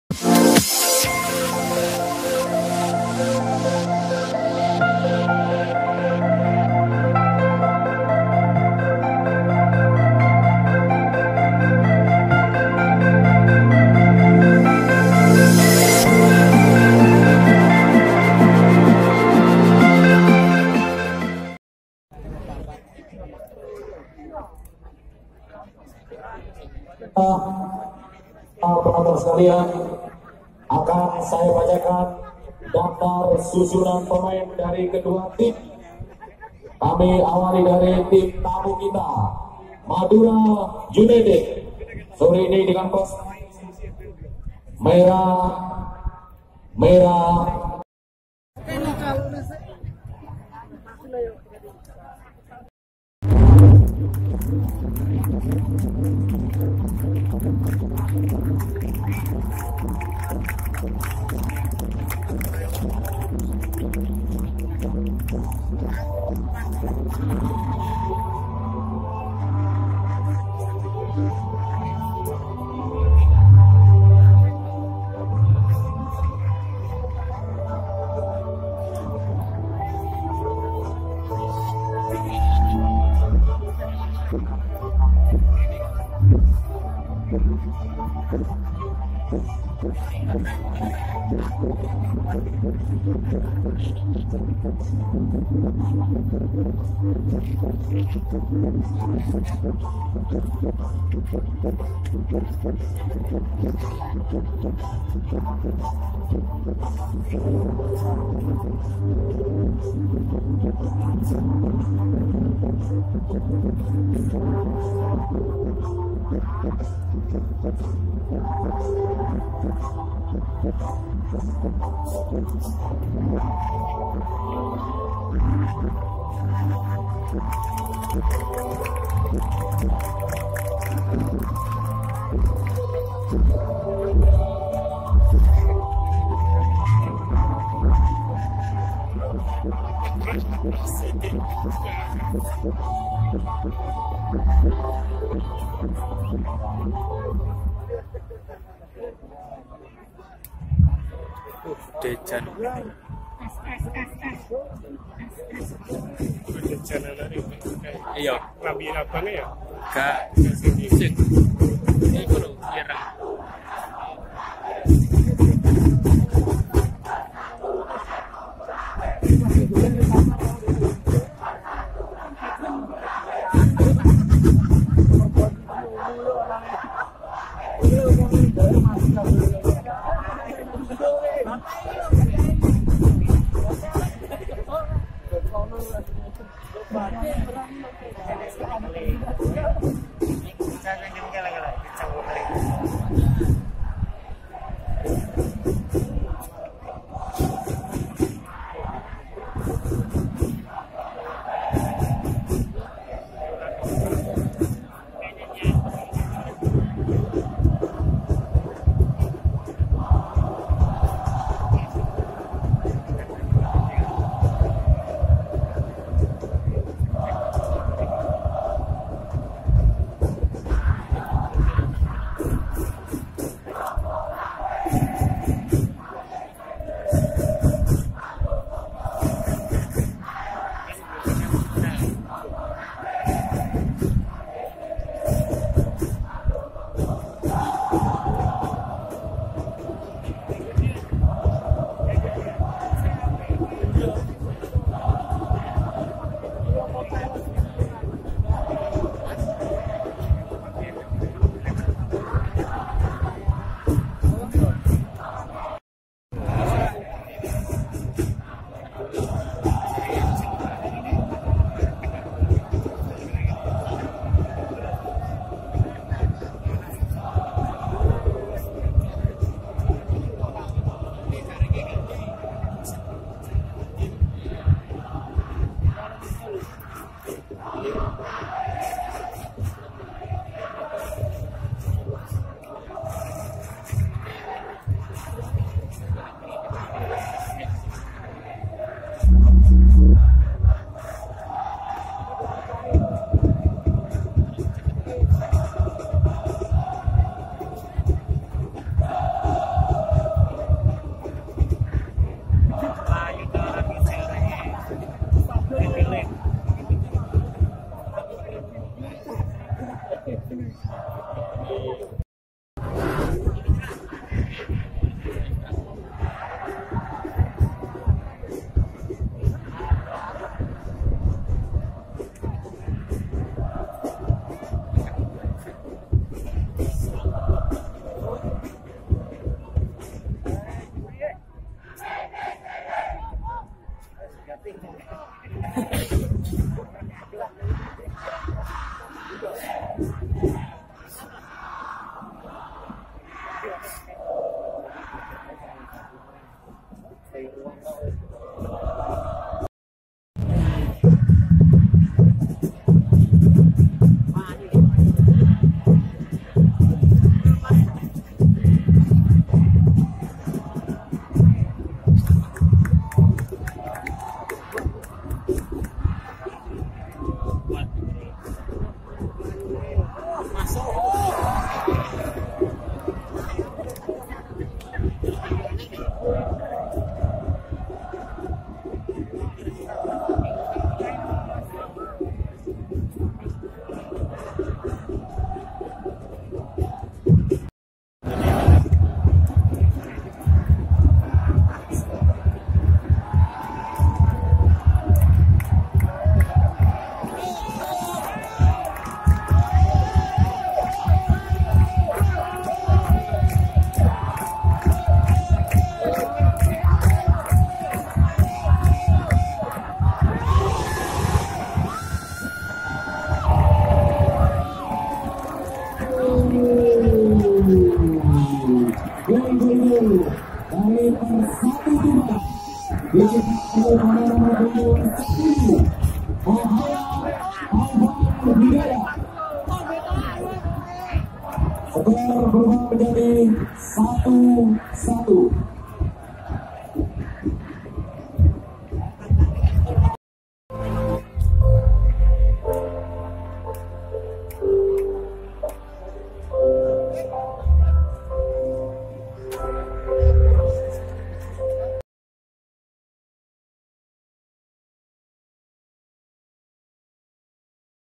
HIGH LIGHT PERSATU FC (TUBAN) 1 VS 2 MADURA UNITED FC 2019
PERSATU TUBAN MADURA UNITED

Highlight Laga Friendly Match Laskar Ronggolawe kalah tipis oleh Laskar Sappe Kerrab. Pertandingan berjalan menarik saling serang. Persatu yang menguasai pertandingan beberapa kali mendapatkan peluang emas. Namun belum bisa berbuah gol.